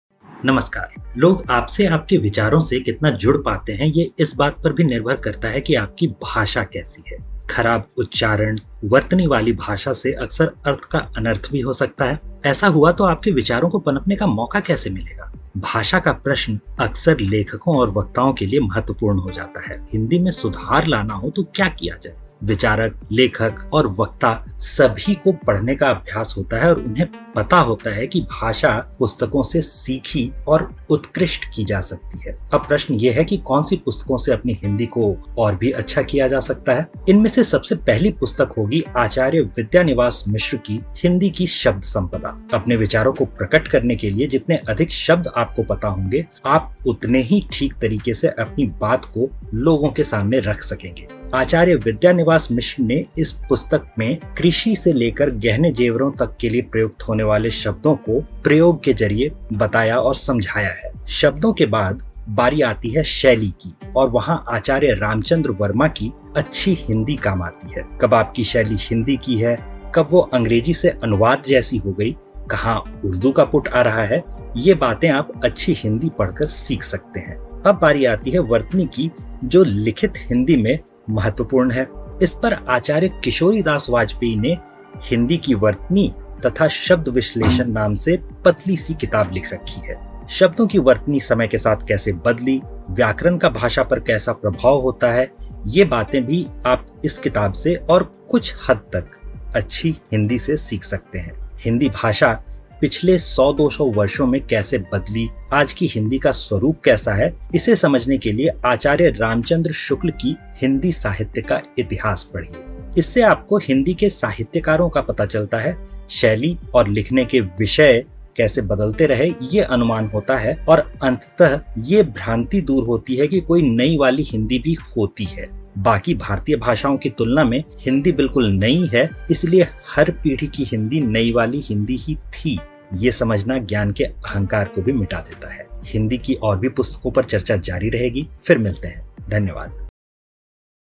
हिंदी में सुधार लाना हो तो क्या किया जाये? - पुस्तक सुझाव
लोग आपसे, आपके विचारों से कितना जुड़ पाते हैं, ये इस बात पर भी निर्भर करता है कि आपकी भाषा कैसी है। खराब उच्चारण-वर्तनी वाली भाषा से अक्सर अर्थ का अनर्थ भी हो जाता है। ऐसा हुआ तो आपके विचारों को पनपने का मौका कैसे मिलेगा? भाषा का प्रश्न अक्सर लेखकों और वक्ताओं के लिए महत्वपूर्ण हो जाता है। प्रश्न ये है कि अगर हिंदी में सुधार लाना हो तो क्या किया जाये? विचारक, लेखक और वक्ता सभी को पढ़ने का अभ्यास होता है और उन्हें पता होता है कि भाषा पुस्तकों से सीखी-और उत्कृष्ट की जा सकती है।

अब प्रश्न है कि कौन सी पुस्तकों से अपनी हिंदी और अच्छी की जा सकती है? इनमें सबसे पहली पुस्तक होगी आचार्य विद्यानिवास मिश्र की “हिंदी की शब्द सम्पदा”। अपने विचारों को प्रकट करने के लिए जितने अधिक शब्द आपको पता होंगे, आप उतने ही ठीक तरीके से अपनी बात लोगों के सामने रख सकेंगे। अपने लेखों में आचार्य विद्यानिवास मिश्र ने इस पुस्तक में कृषि से लेकर गहने-जेवरों तक के लिए प्रयुक्त होने वाले शब्दों को प्रयोग के जरिये बताया है। शब्दों के बाद बारी आती है शैली की और वहाँ आचार्य रामचन्द्र वर्म्मा की “अच्छी हिंदी” काम आती है। कब आपकी शैली हिंदी की है, कब वो अंग्रेजी से अनुवाद जैसी हो गयी, कहाँ उर्दू का पुट आ रहा है, ये बातें आप “अच्छी हिंदी” पढ़कर सीख सकते हैं।

अब बारी आती है वर्तनी की जो लिखित हिंदी में महत्वपूर्ण हो जाता है। इसपर आचार्य किशोरीदास वाजपेयी ने “हिंदी की वर्तनी तथा शब्द-विश्लेषण” नाम की पतली सी किताब लिखी है। शब्दों की वर्तनी समय के साथ कैसे बदली, व्याकरण का भाषा पर कैसा प्रभाव होता है, ये बातें भी आप इस किताब से और कुछ हद तक “अच्छी हिंदी” से सीख सकते हैं। हिंदी भाषा पिछले सौ-दो सौ वर्षों में कैसे बदली, आज की हिंदी का स्वरुप कैसा है, इसे समझने के लिए आचार्य रामचंद्र शुक्ल की “हिंदी साहित्य का इतिहास” पढ़िए। इससे आपको हिंदी के साहित्यकारों का पता चलता है, शैली और लिखने के विषय कैसे बदलते रहे ये अनुमान होता है और अंततः ये भ्रान्ति दूर होती है कि कोई “नयी वाली हिंदी” भी होती है। बाकि भारतीय भाषाओँ की तुलना में हिंदी बिलकुल नई है, इसलिए हर पीढ़ी की हिंदी “नयी वाली हिंदी” ही थी, ये समझना ज्ञान के अहंकार को मिटाता है।

आगे हिंदी की और भी पुस्तकों पर चर्चा जारी रहेगी। फिर मिलते हैं!
अपने हिंदी भाषा के प्रेम को प्रफुल्लित करने के लिए इन पुस्तकों को यहां से क्रय कर सकते हैं-